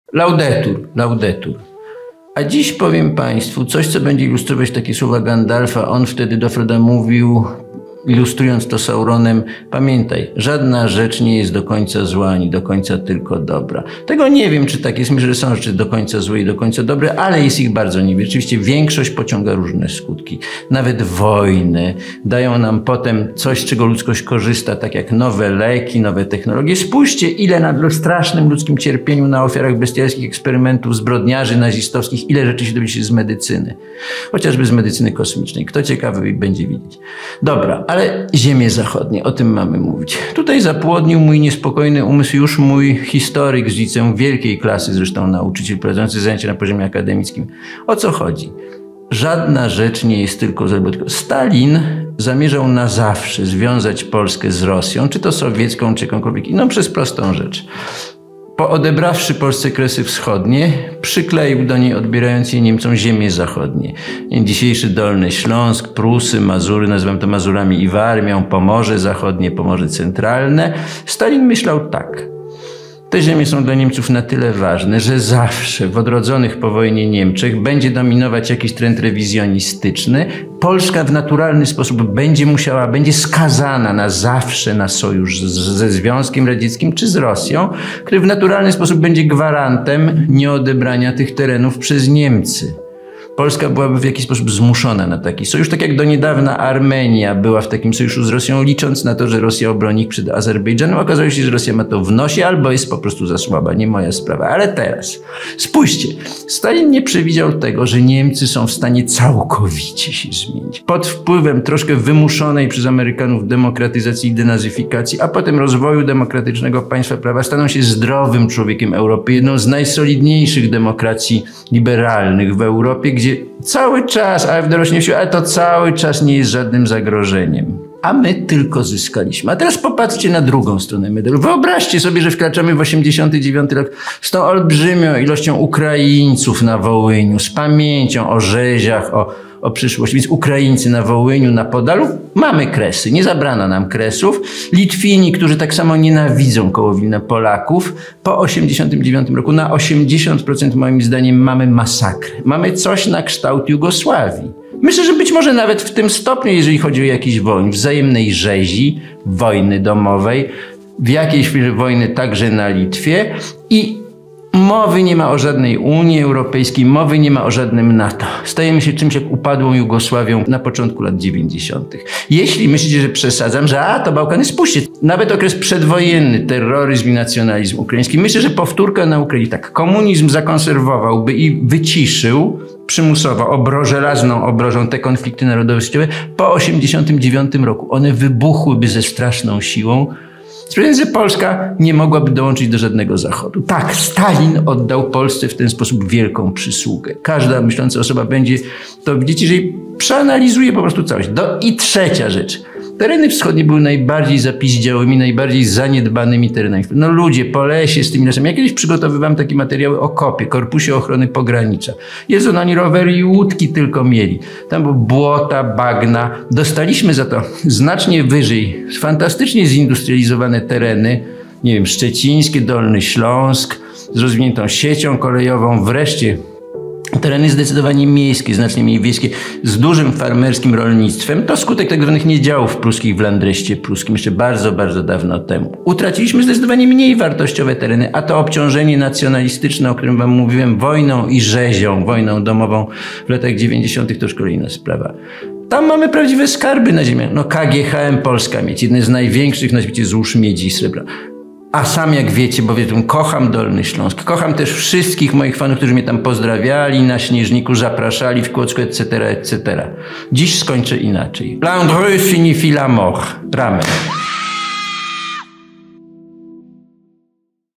Nie ma tego złego czyli przysługa Stalina
Maciej Gos o tym dlaczego utrata kresów wschodnich mogła okazać się wcale nie taka straszna